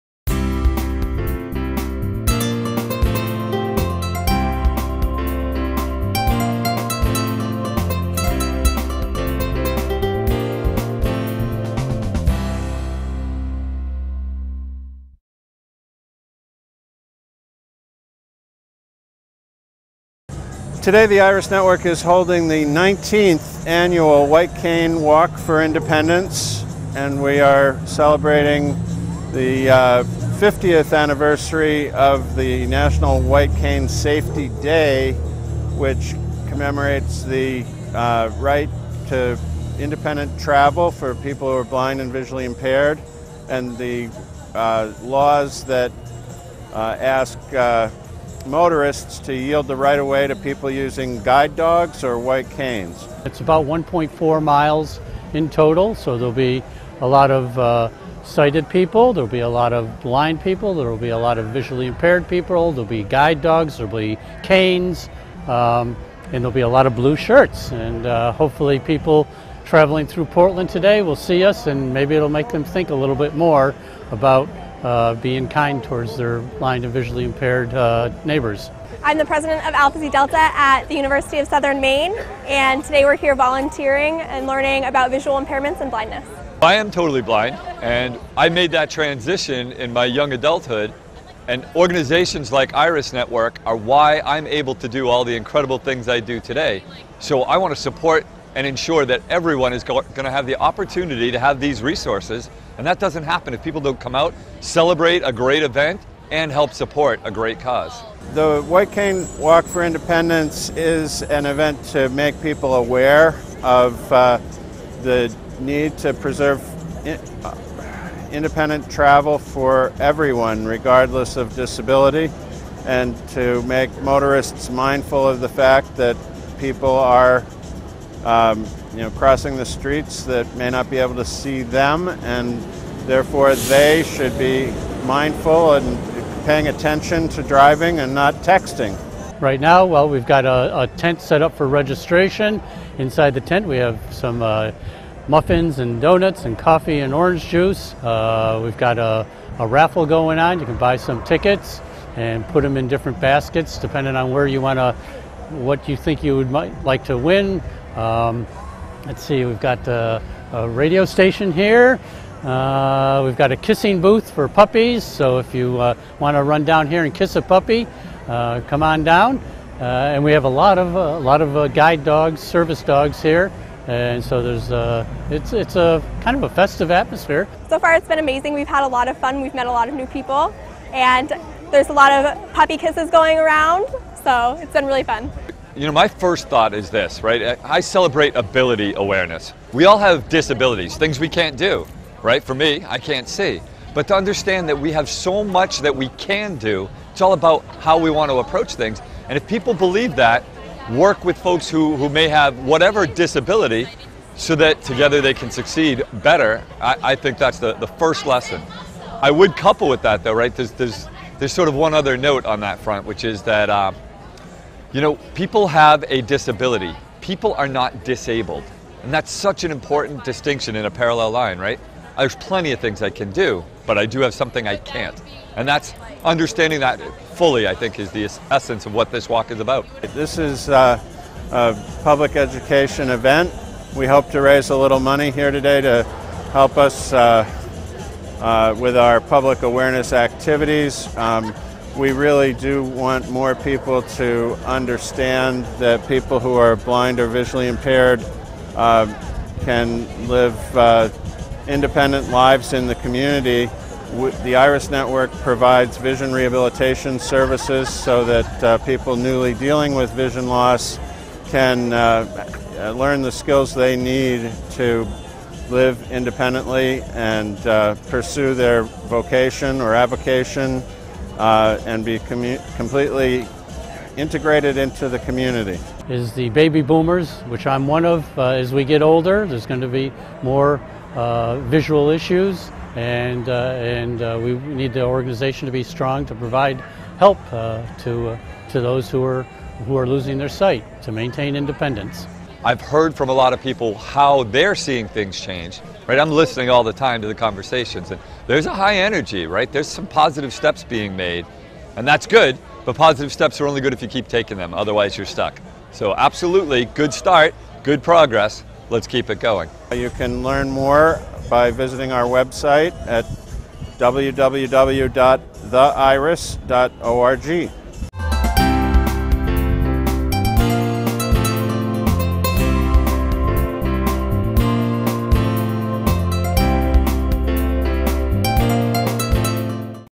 Iris Network White Cane Walk
Join the Iris Network on their 2014 White Cane Walk in Portland